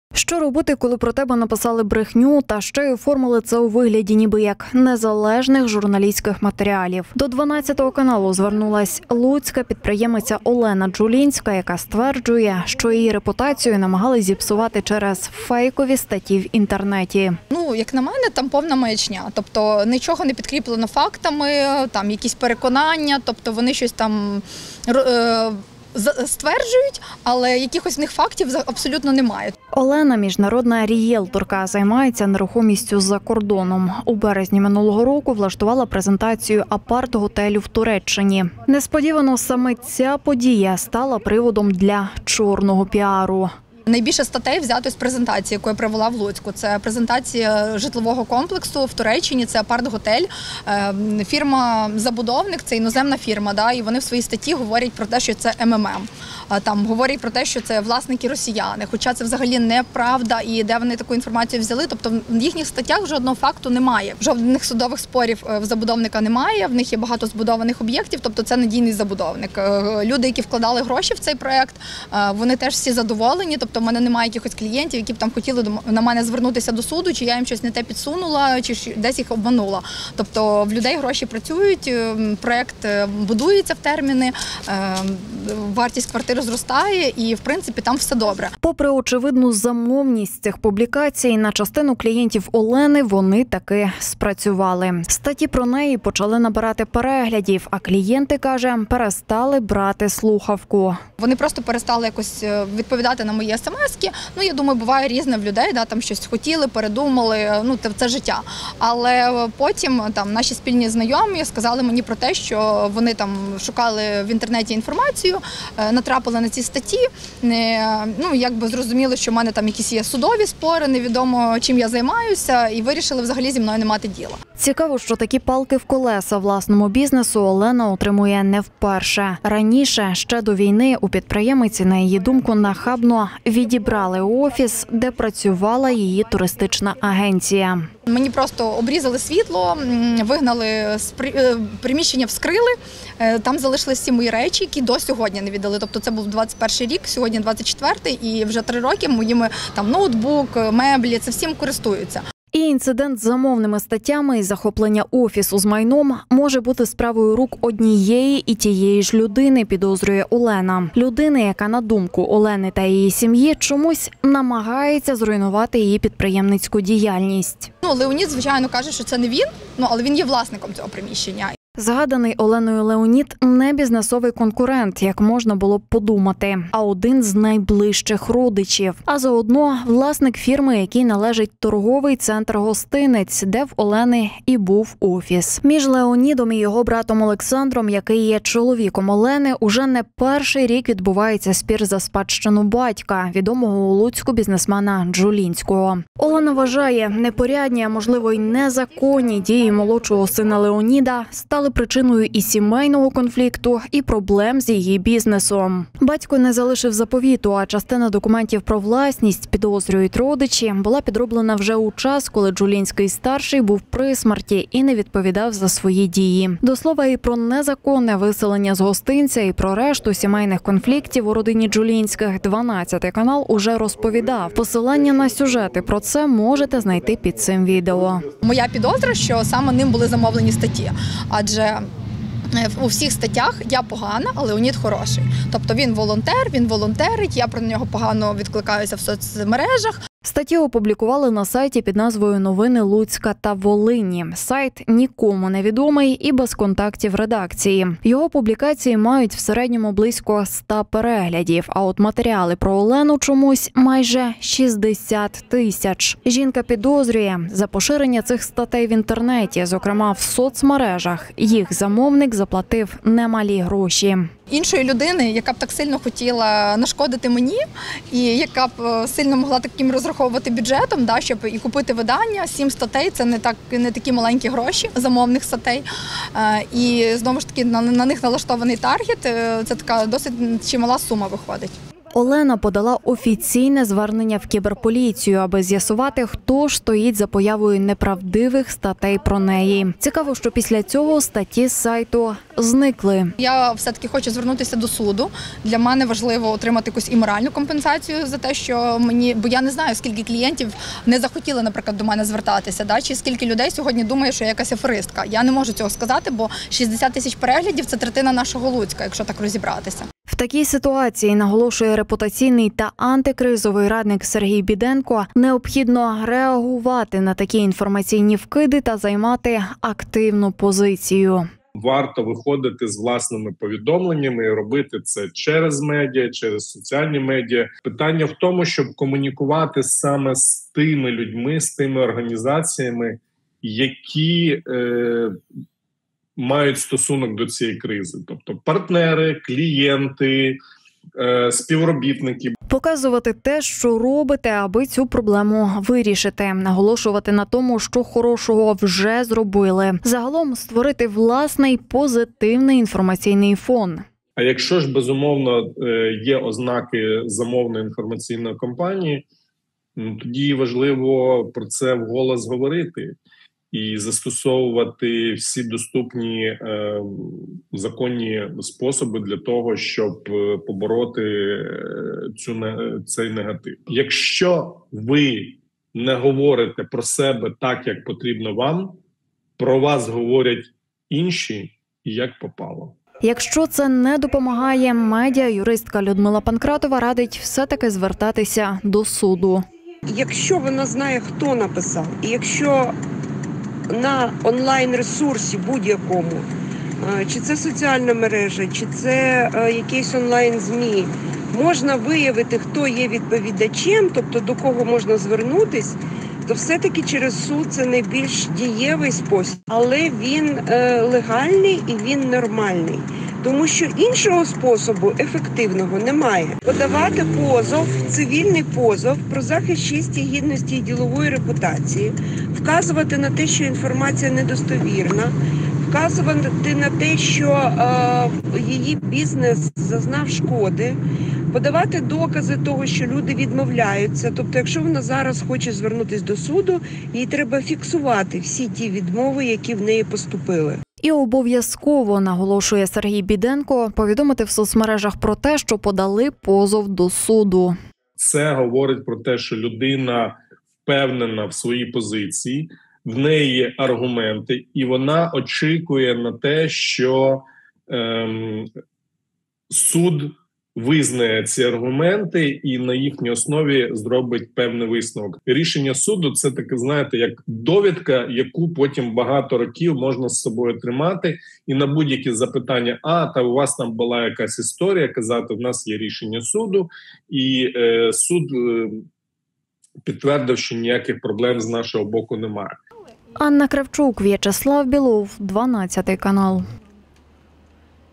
🤯 Луцьку підприємицю Олену Джулінську оббрехали в інтернеті. Вона підозрює …родичів
Що робити, коли про тебе написали брехню, та ще й оформили це у вигляді ніби як незалежних журналістських матеріалів? До 12 каналу звернулась луцька підприємиця Олена Джулінська, яка стверджує, що її репутацію зіпсували брехливі статті в інтернеті.